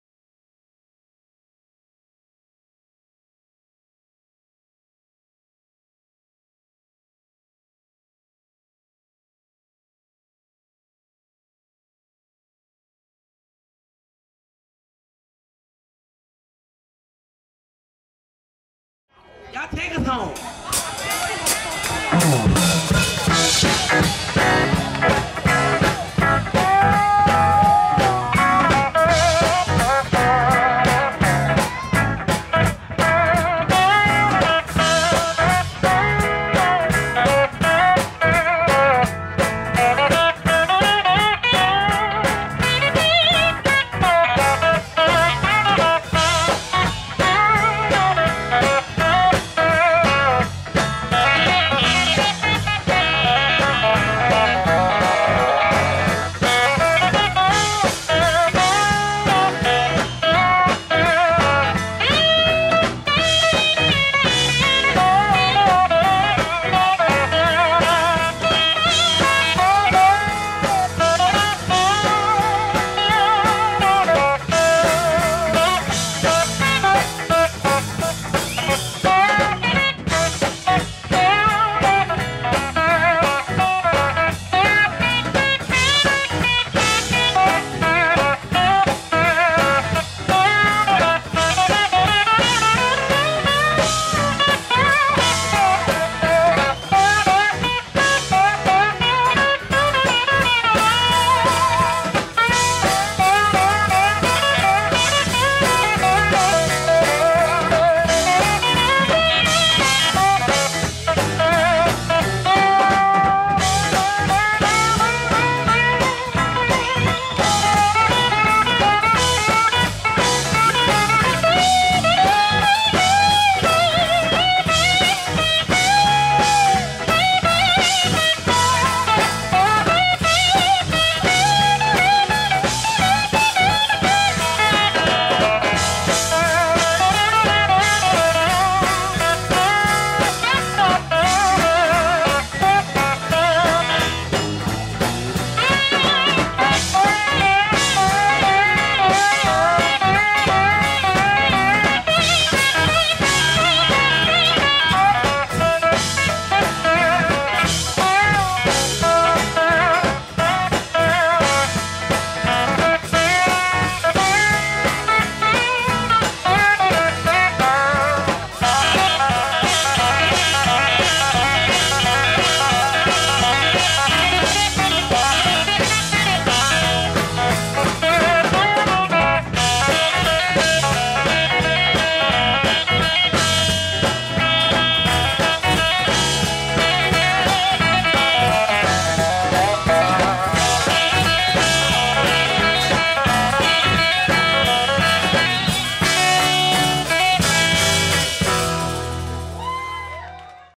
Sacred Steel BBQ -Demetrius Beasley
The young guns of Sacred Steel really put on quite an exhibition at the Sacred Steel BBQ & Concert. Demetrius Beasley of Cleveland, Ohio is on the Steel Guitar, Thaddaeus Washington is on Lead Guitar, Del Ray Grace Jr. is on Bass Guitar, and Chris Pope is on Drums.